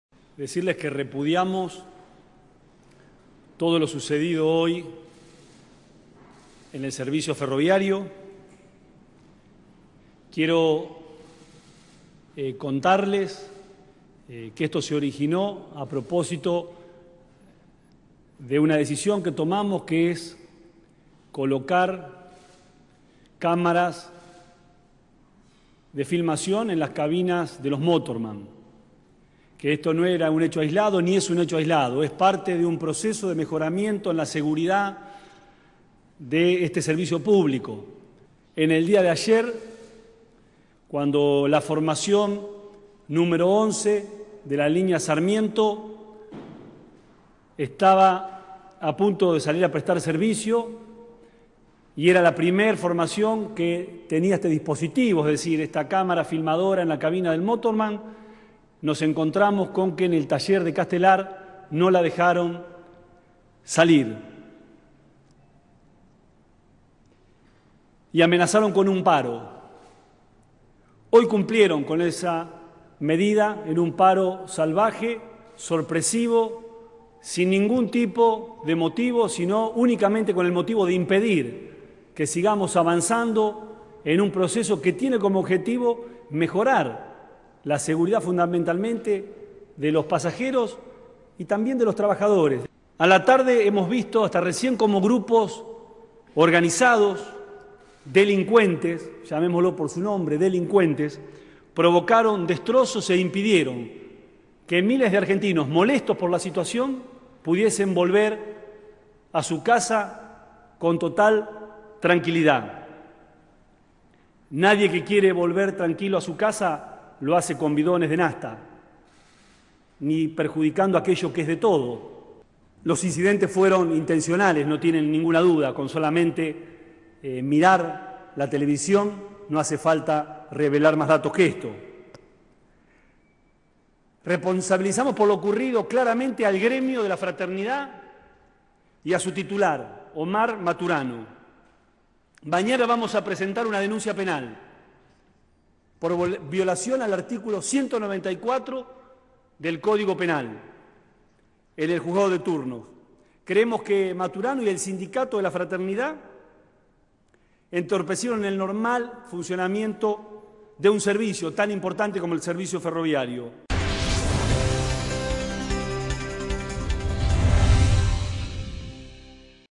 Randazzo: "Presentaremos una denuncia penal contra La Fraternidad"
El ministro del Interior y Transporte, Florencio Randazzo, calificó como "salvaje y sorpresivo" el paro de los maquinistas ferroviarios y dijo que
mañana se presentará "una denuncia penal contra el gremio La Fraternidad y su
titular, Omar Maturano".
    El ministro remarcó que el paro se originó por la negativa de los maquinistas a que se coloquen cámaras en las cabinas de
conducción de las formaciones.